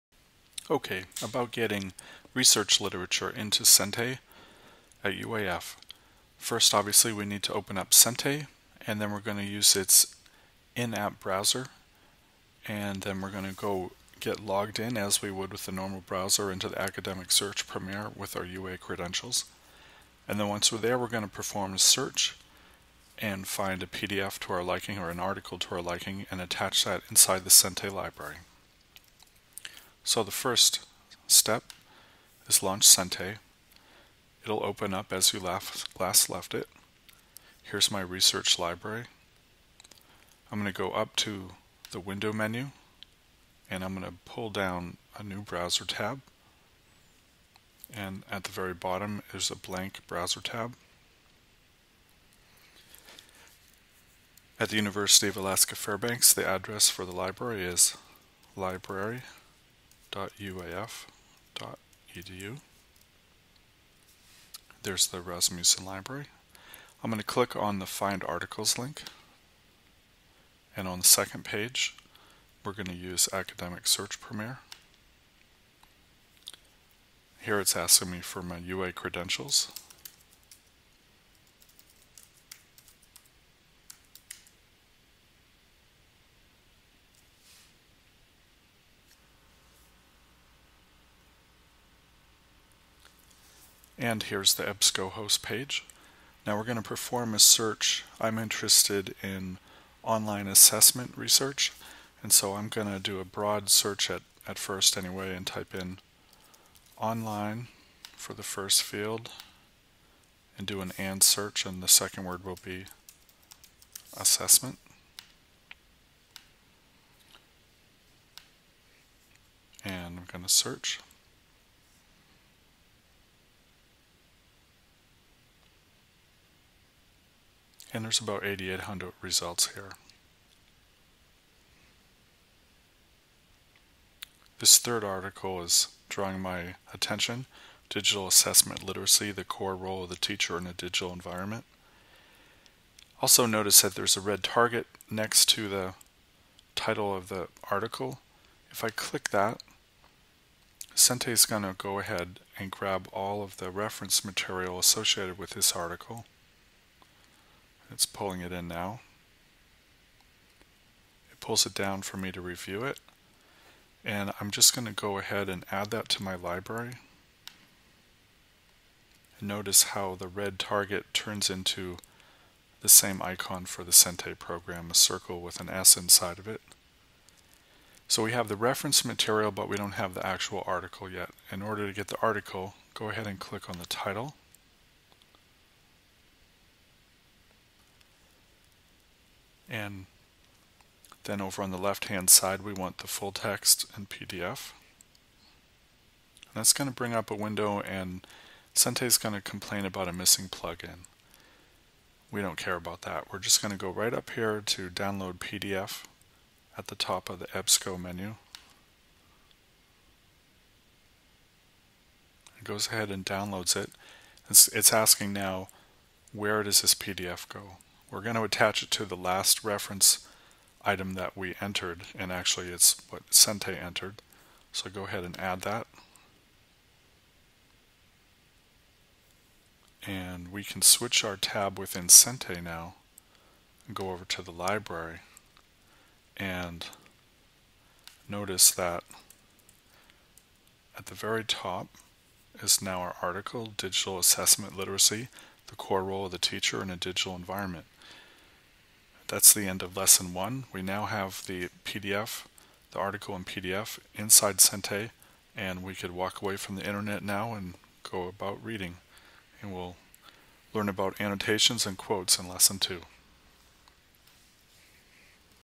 Retrieving Research Articles at UAF with Sente
This screencast is part of an assignment for ED 432 at the University of Alaska Fairbanks. Here I show how to retrieve reference articles from the UAF Library with Sente.